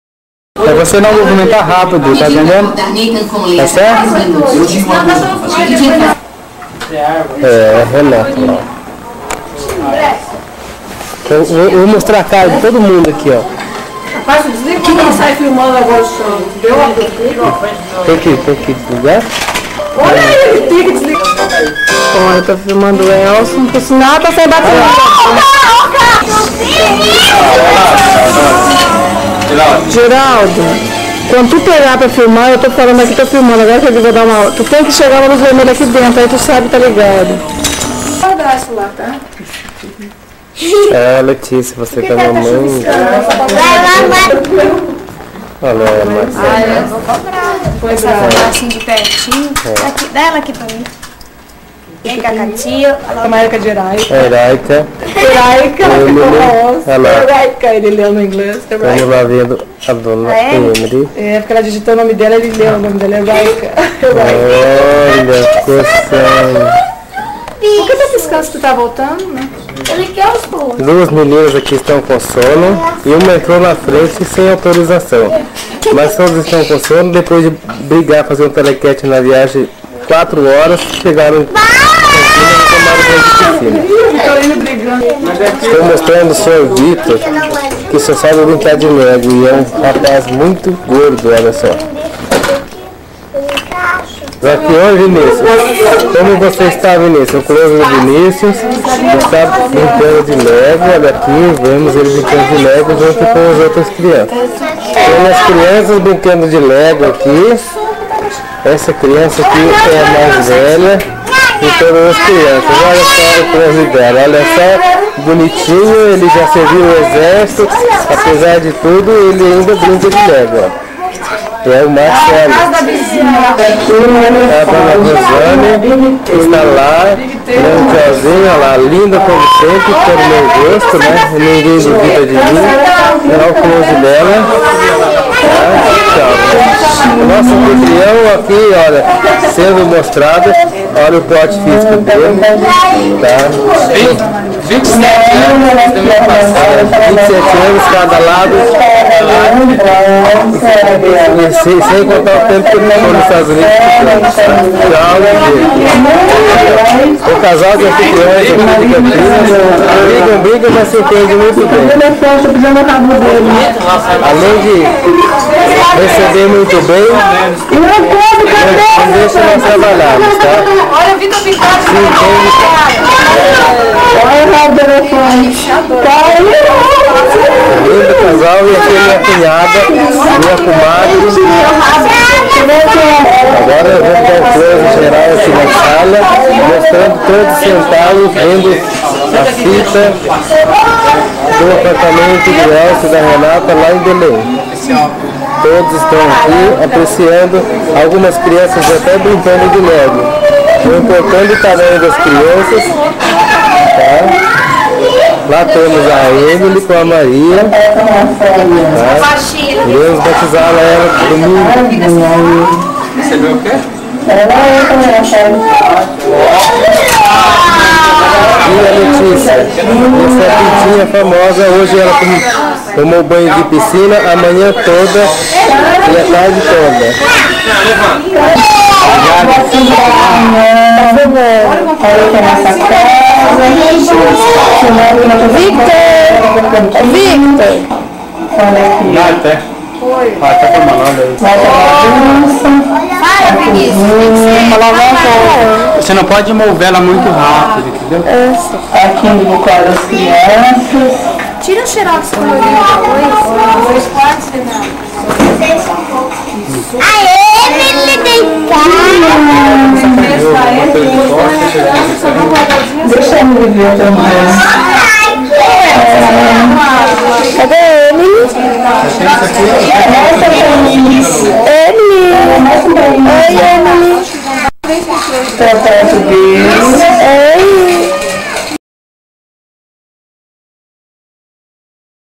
356 Tubarc - Campinas Geraldo Gravando
Geraldo aprendendo a gravar com a filmadora 8MM